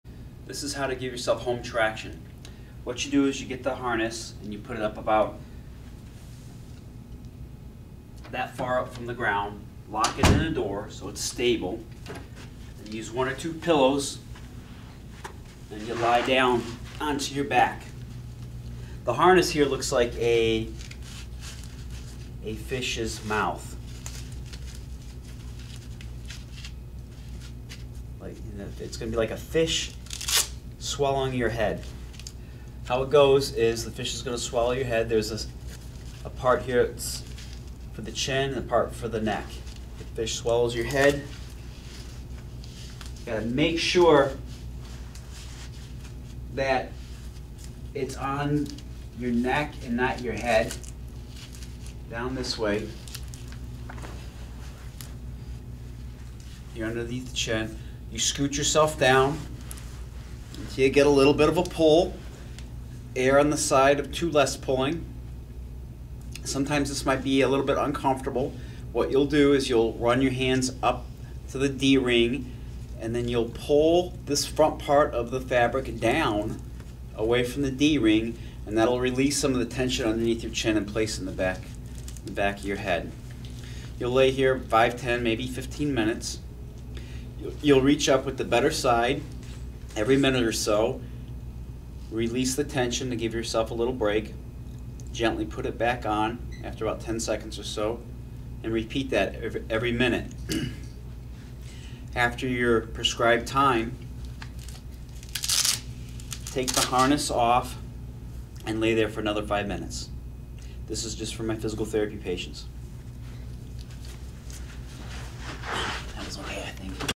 SELF TRACTION
This is a self neck traction with a elastic tube and a harness. This video is to compliment the instructions that were given to my physical therapy patients.